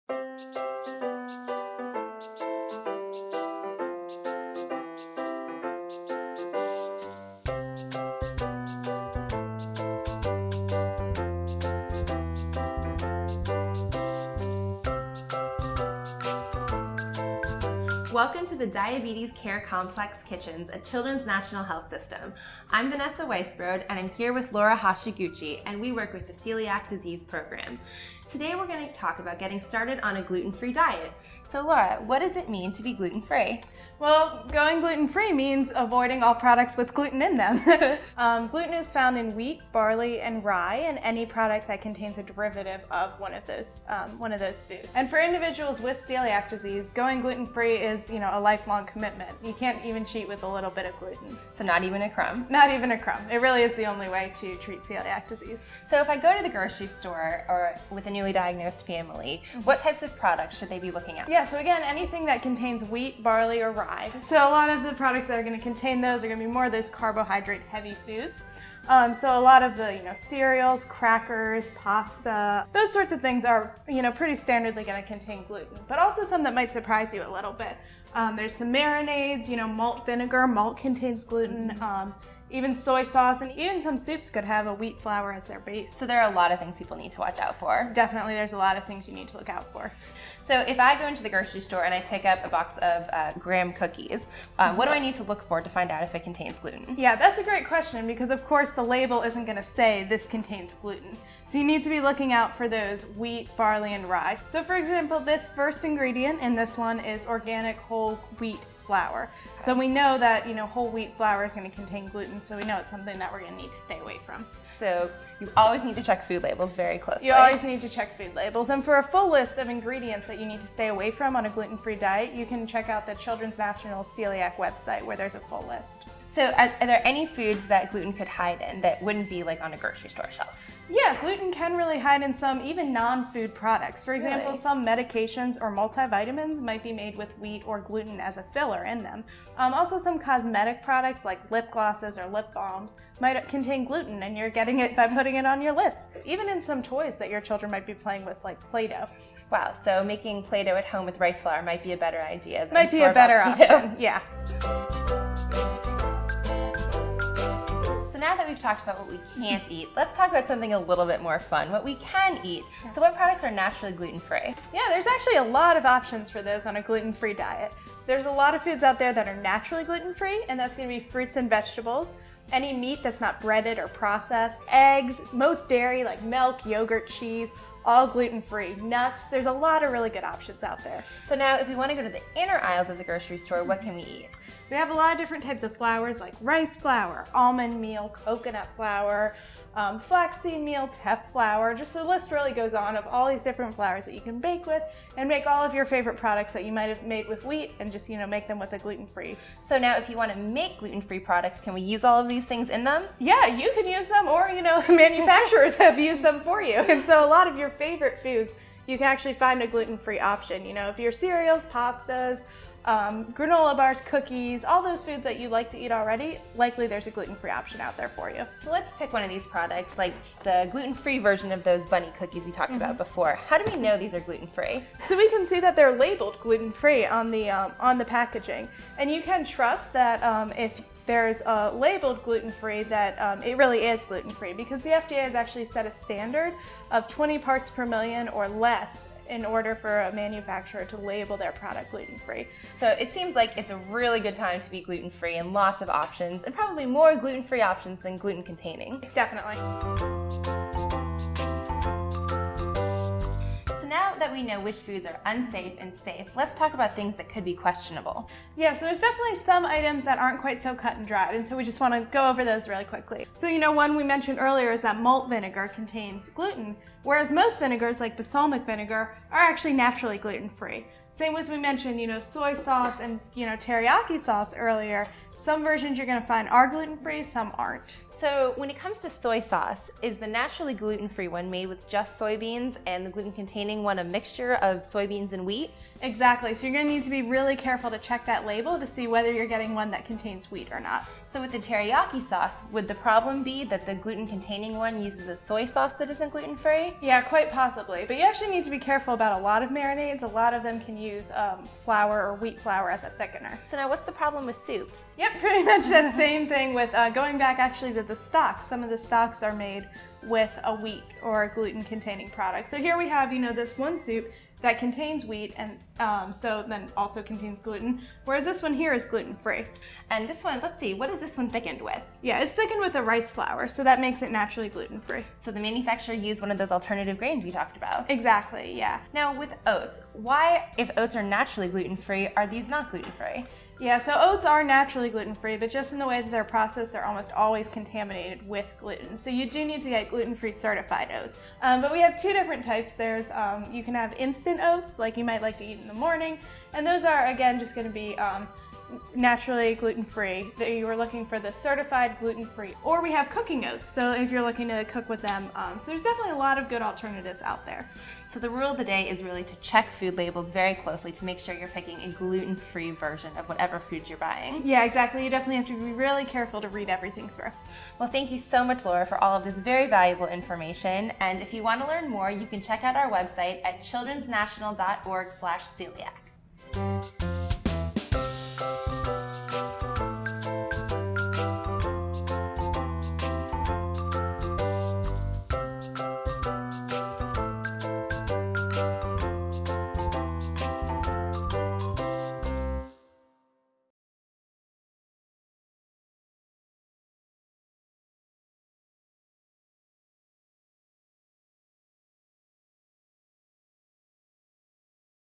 Getting Started on a Gluten-free Diet
Children's National Celiac Disease Program Dietitian Laura Hashiguchi introduces families to the gluten-free diet by exploring safe, unsafe, and questionable foods. You'll learn all about reading food labels and how the new Food and Drug Administration food labeling laws affect products on grocery store shelves.